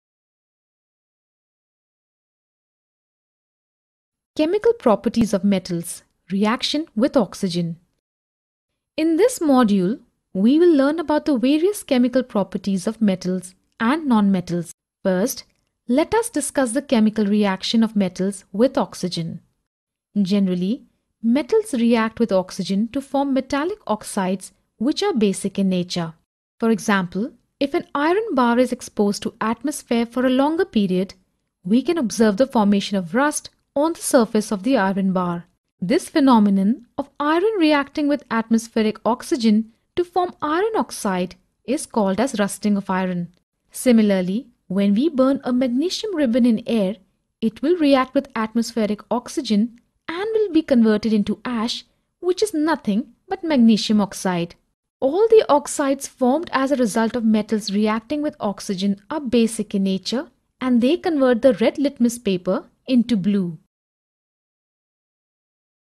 NCERT CBSE Class 8 Science Chapter 4 Materials- Metals and Non-Metals Part 1
In this video “Chemical Properties of Metals- Reaction with Oxygen” has been explained according to NCERT CBSE Class 8 Science Chapter 4 “Materials- Metals and Non-Metals”. for complete free access to the course